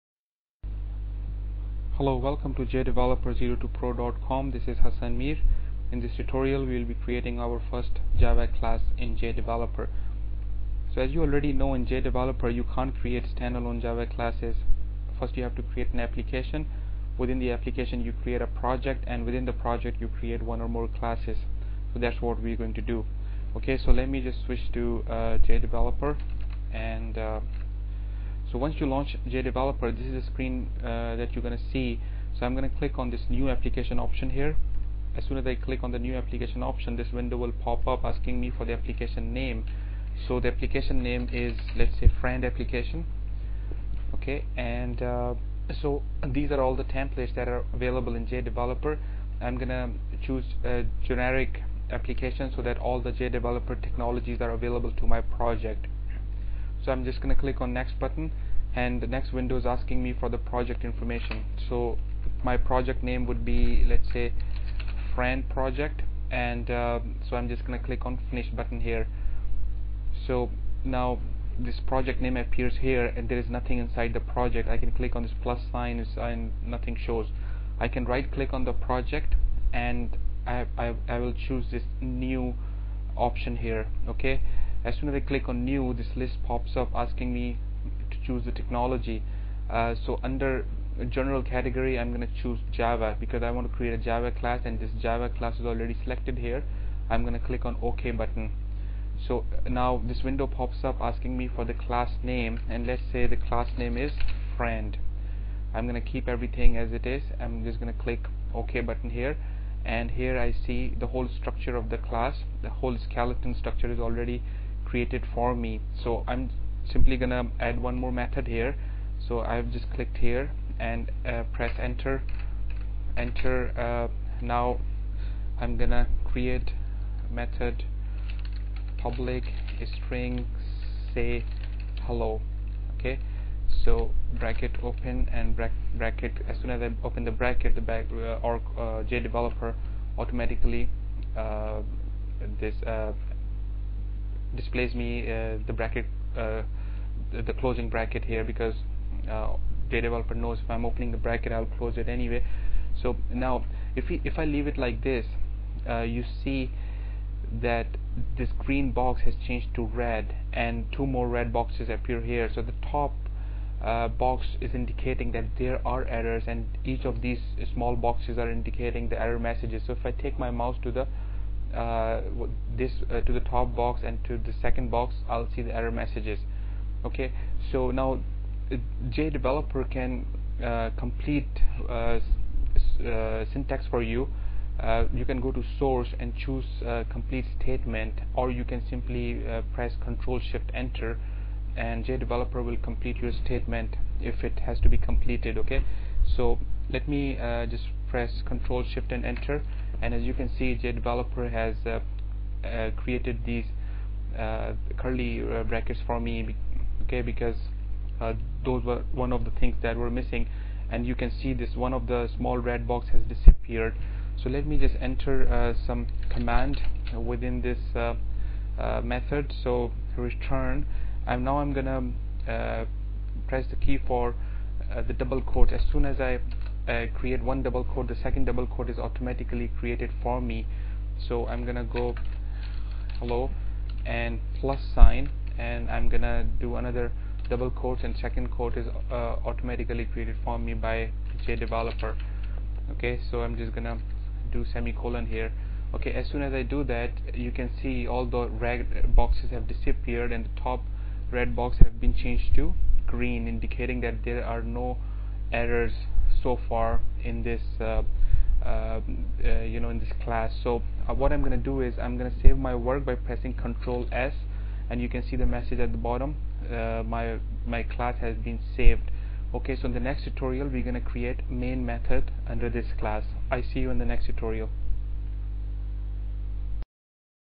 Oracle Developer - Creating Your First Java Class
In this tutorial you will be creating your first Java Class in JDeveloper. You will also learn to use "Complete Statement" feature of JDeveloper.